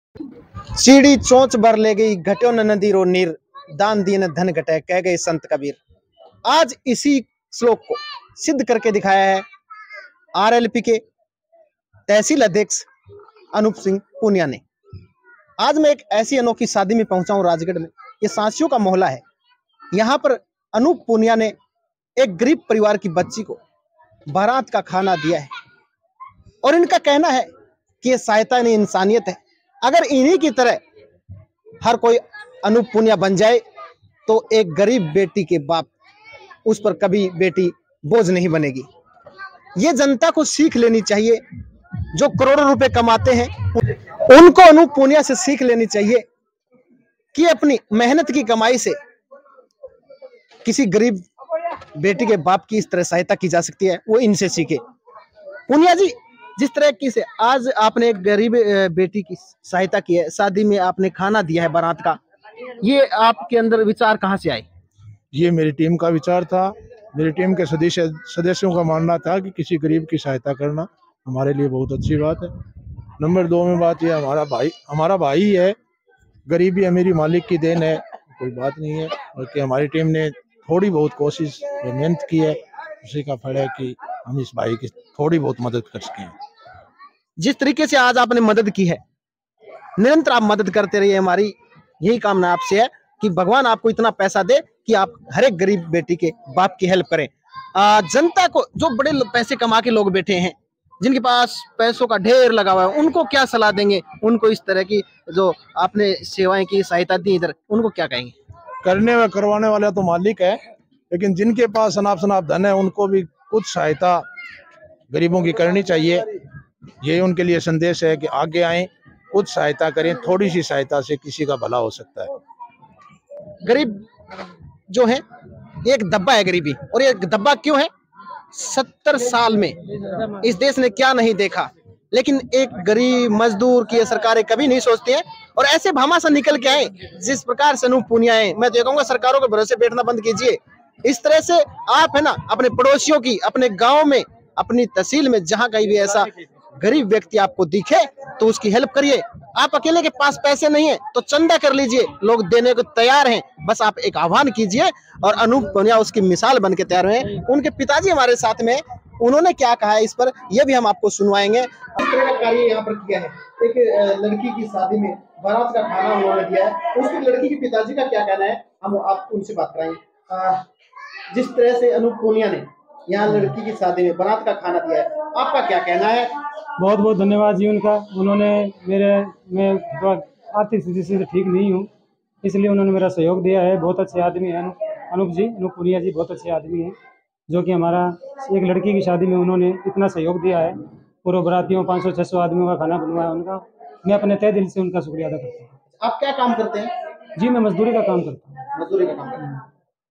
RLP के तहसील अध्यक्ष Anoop poonia ने की गरीब परिवार की सहायता। दिया पूरी बारात का खाना।
अनूप पुनिया ने गरीब लड़की कि शादी में दिया पुरी बरात को खाना